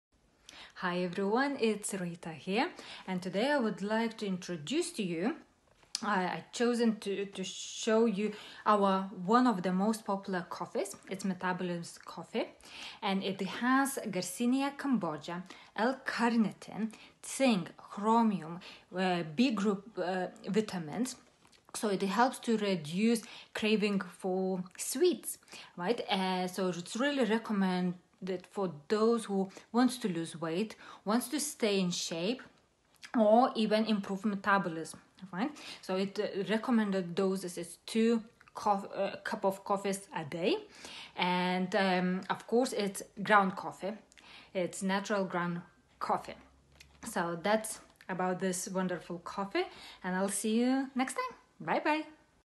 FM World Product - Aurile Metabolism Coffee
This 250g Aurile Metabolism - Ground Coffee with Garcinia Cambogia Extract and L-Carnitine. Counting calories? Or maybe you just feel that your metabolism has considerably slowed down? Wake it up with a cup of delicious coffee, enhanced with zinc, chrome, L-carnitine, garcinia cambogia extract, and vitamin B. It’s an excellent and most of all, safe supplement of your slimming and detoxification processes. With a proper diet and a little bit of sport it will make you feel exceptionally light.
→ Ingredients:
ground coffee, Garcinia cambogia extract, L-carnitine, tartrate, zinc lactate, chromium chloride, vitamins: niacin, pantothenic acid, riboflavin, biotin.

Total weight : 289.0 [g]